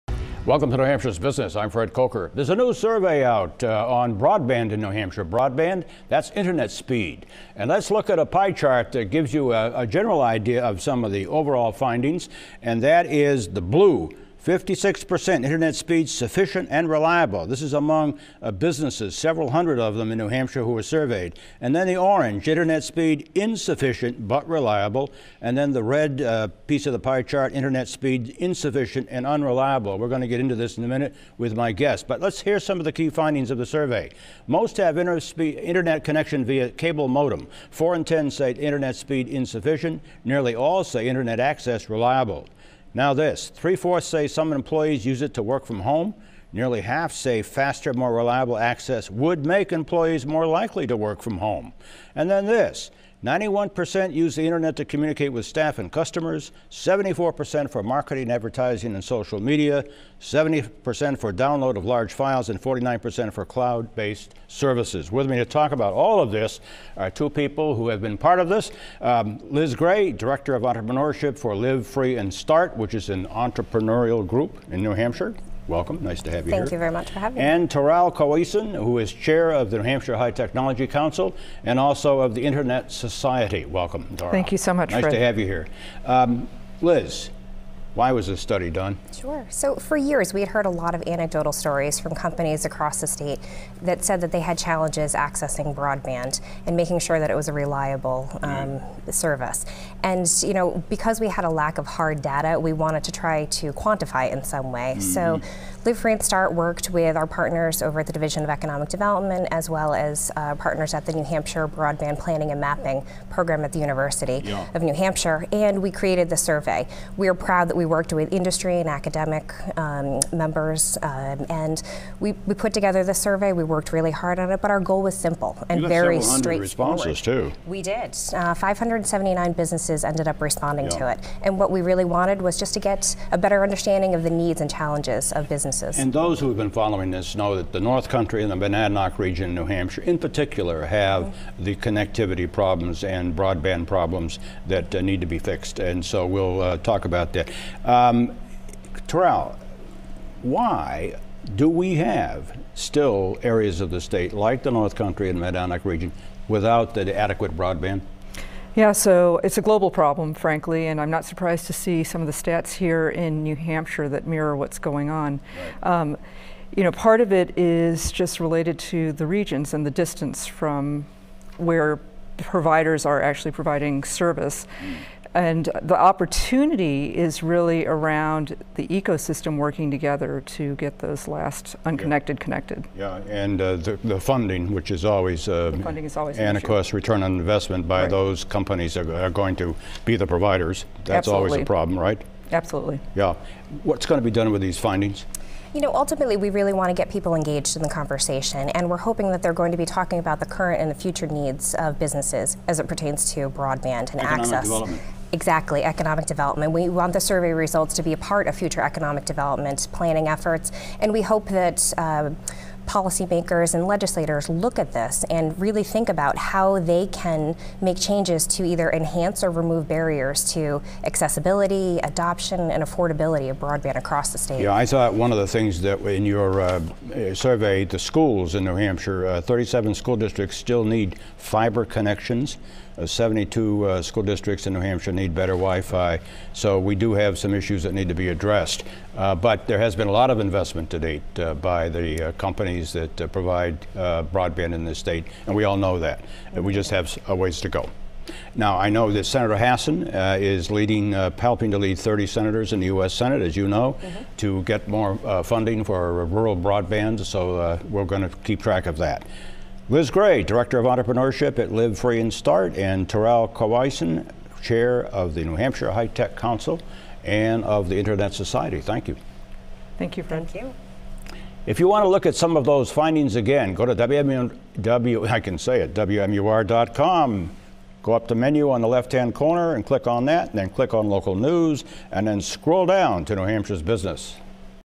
Internet speed insufficient for 4 in 10 NH businesses surveyed
Fred Kocher discusses a new survey that revealed internet speeds at NH businesses.